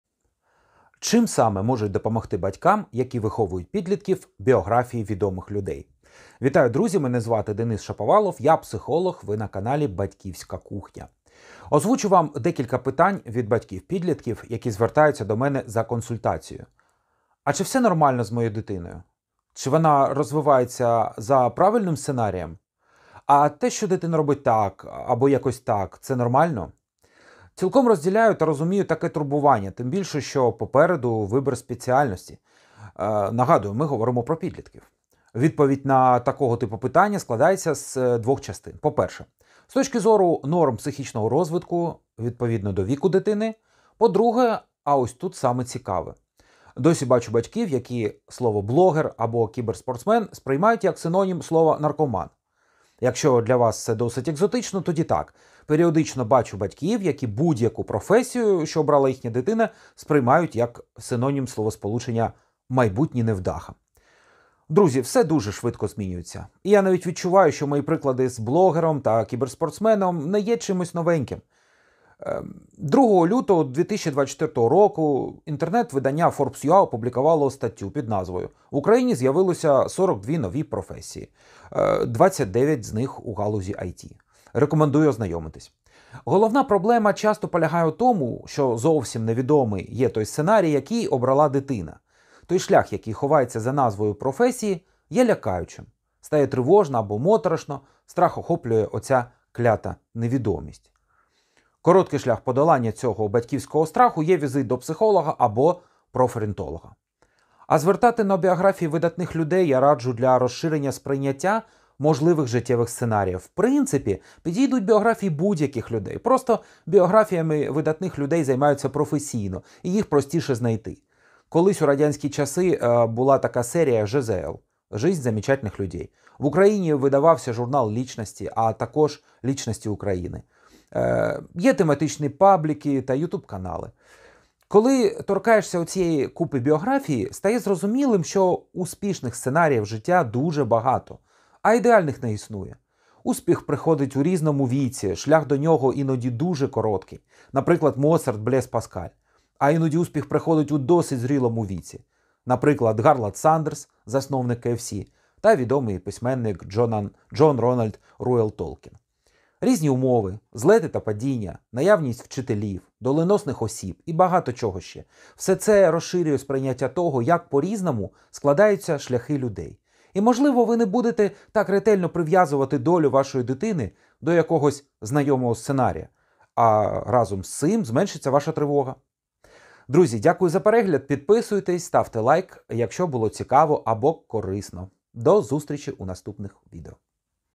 Біографії відомих людей. Для батьків підлітків.
У цьому відео розповідаю про те, чому корисно батькам підлітків звертати на біографії відомих людей.
Про автора відео: Шаповалов Денис. Психолог. Спікер та автор книг з дитячо-батьківських та сімейних відносин. Практичний досвід із 2007 року.